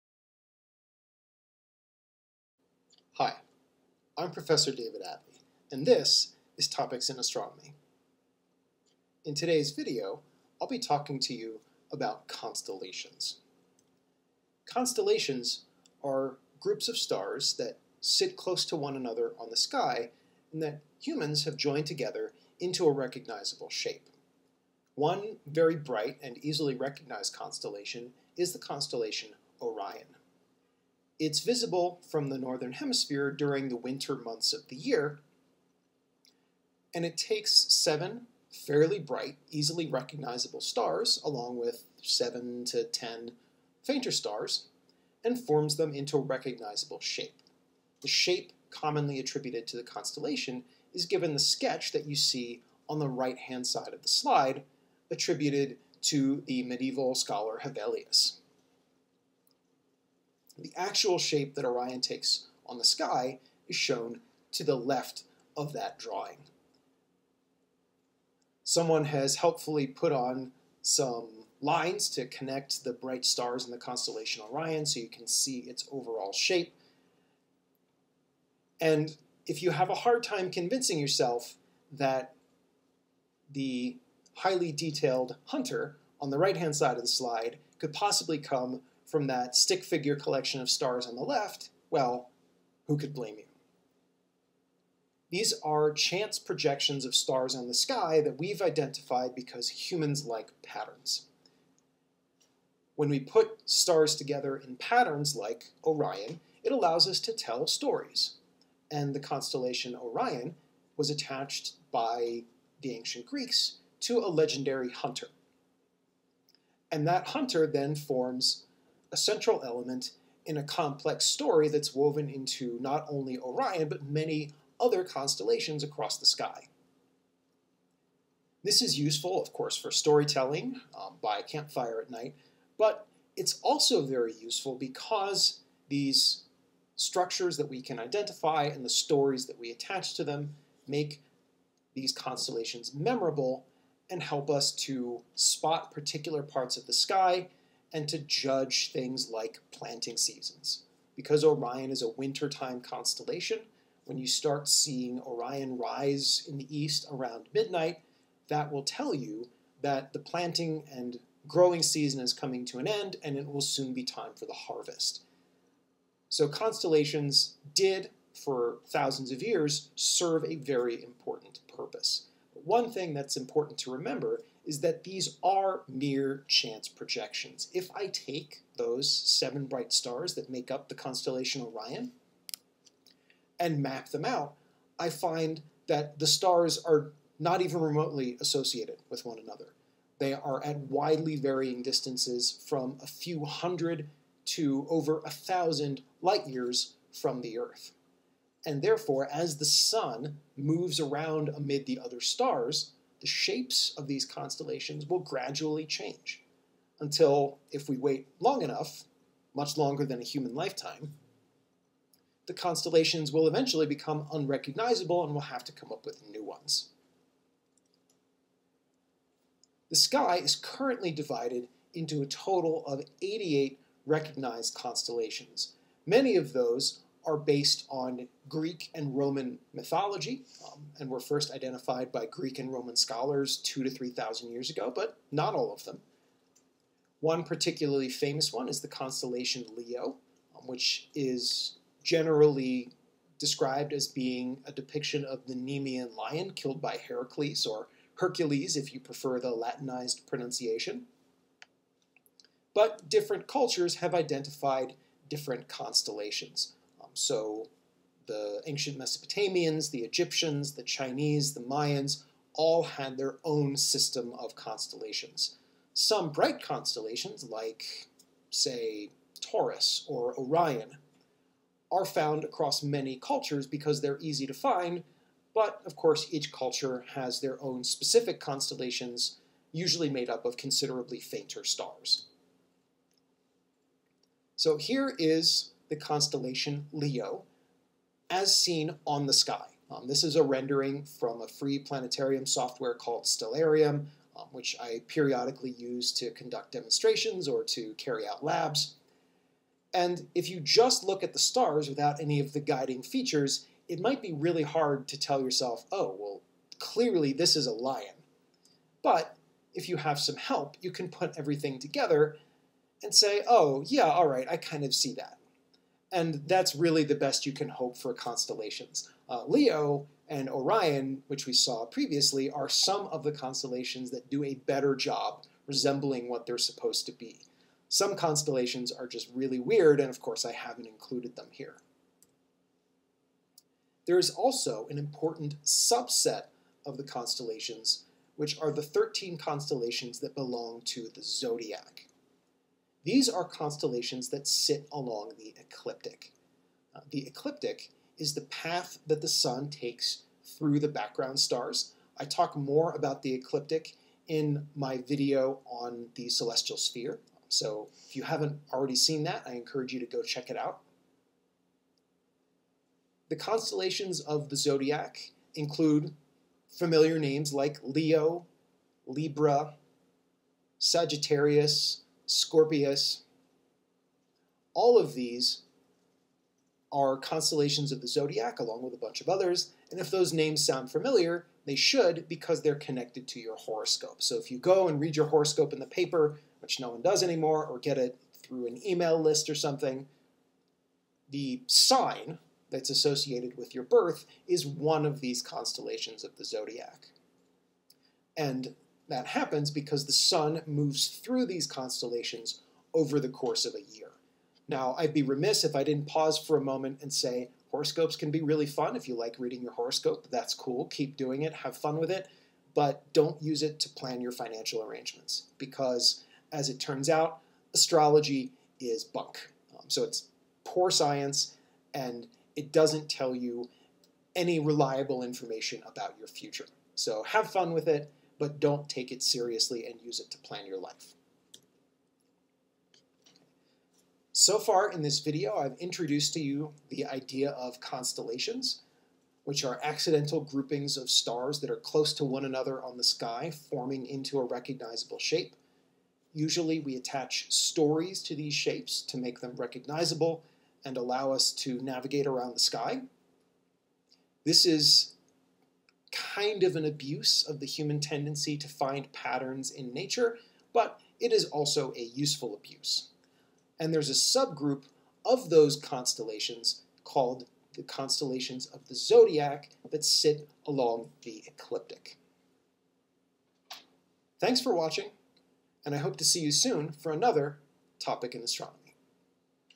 Constellations
A discussion of constellations.  Why do we have them, how are they useful, and just how real are they?